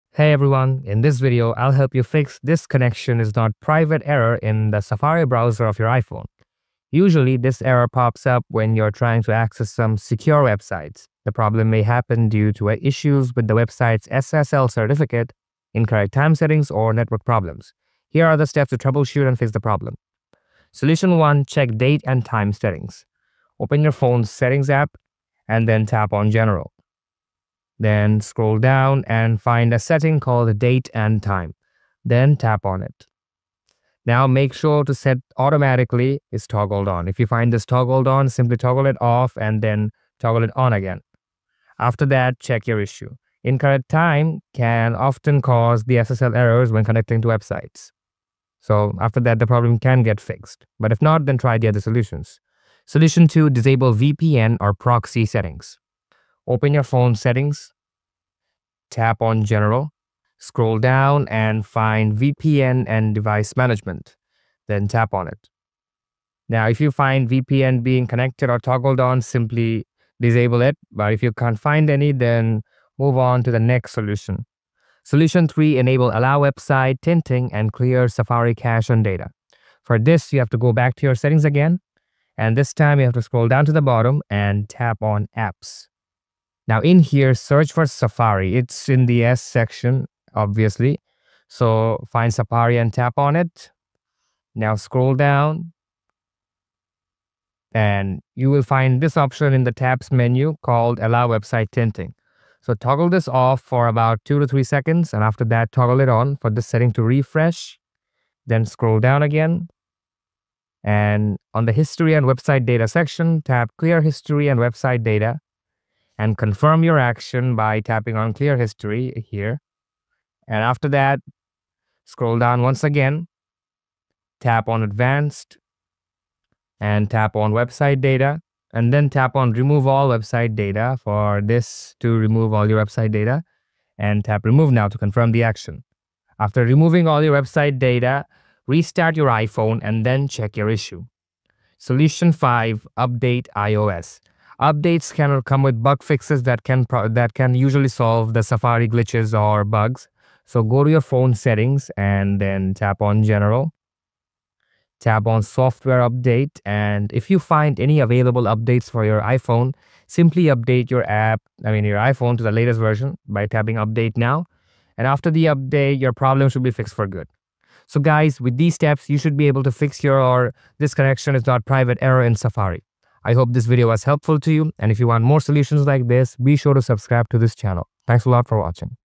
How to Fix “This Connection Is Not Private” in Safari on iPhone | Safari Security Warning Fix
If you’re seeing the “This Connection Is Not Private” warning in Safari on your iPhone, don’t worry! It’s a common issue, but the good news is that it’s easy to fix. In this video, I’ll walk you through a simple solution to remove this remove this connection is not private error on your iPhone and get back to browsing safely. By the end of this video, you’ll be able to fix this connection not private error on your own and browse the web without any interruptions.